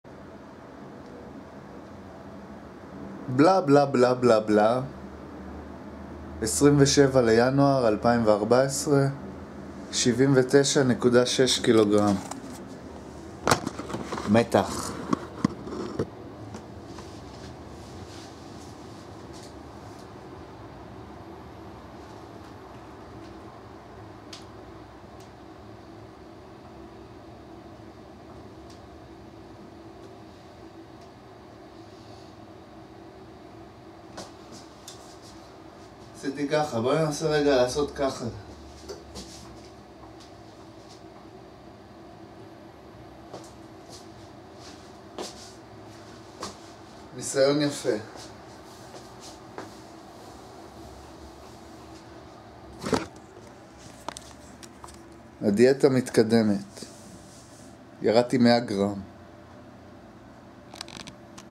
1907 - אבא קריר עושה מתח 27 27.1.2014
יום 27 של 2014 = מתח 27 והפעם 79.6 קילו